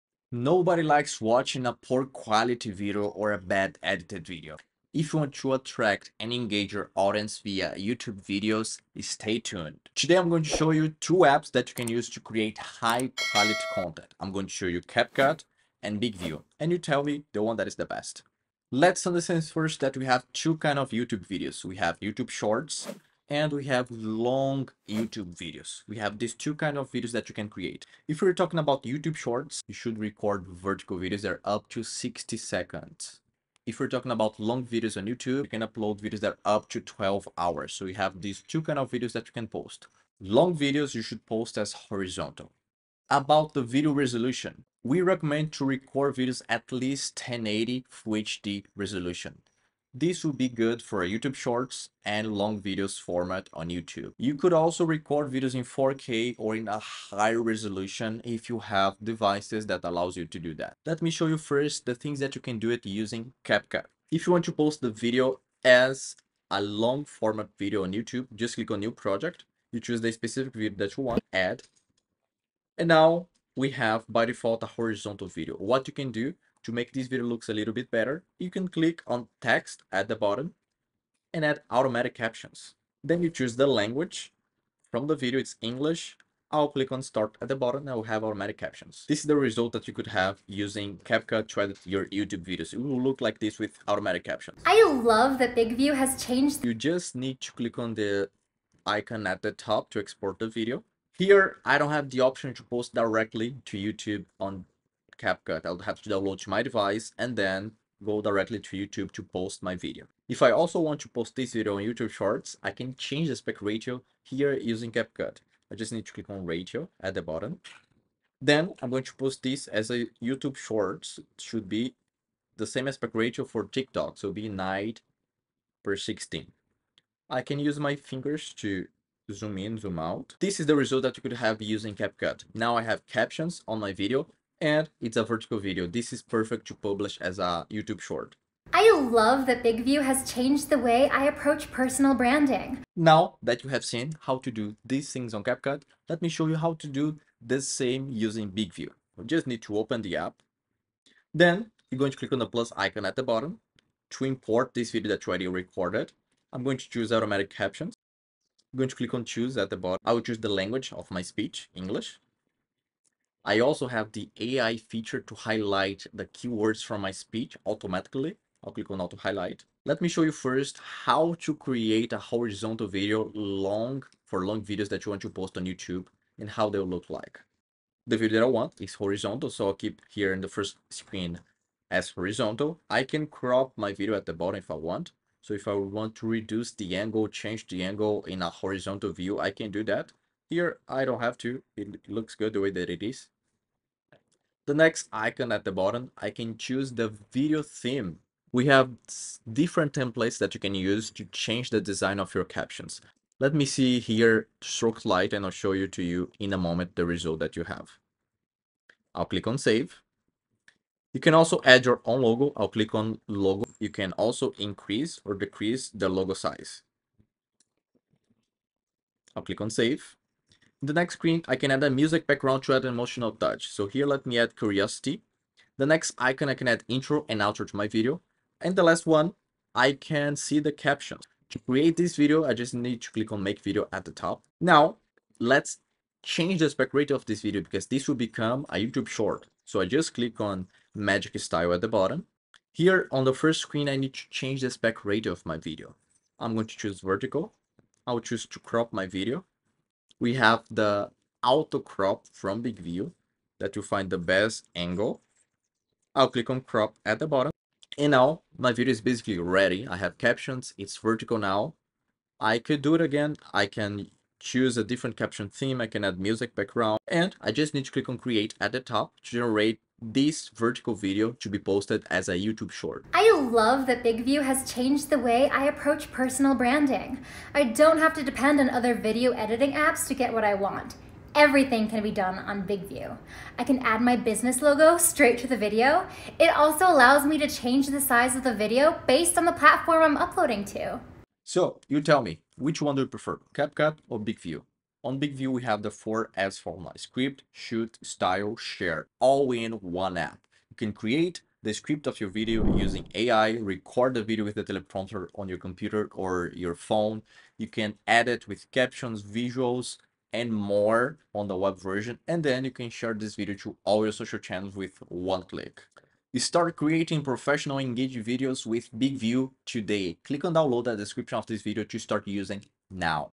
How to Create Stunning YouTube Videos Easily: CapCut vs. BIGVU | Ultimate Editing Showdown!
Struggling with video quality and editing on YouTube? Watch this comprehensive comparison of CapCut and BIGVU!

Discover the best apps for creating high-quality content, whether it's YouTube shorts or long-form videos. Learn step-by-step editing techniques for both apps, including adding automatic captions, adjusting resolutions, and optimizing for YouTube Shorts.

Plus, find out how BIGVU revolutionizes personal branding with its all-in-one features, AI-driven script creation, and seamless social sharing capabilities. Choose your editing champion and elevate your YouTube game today! 🚀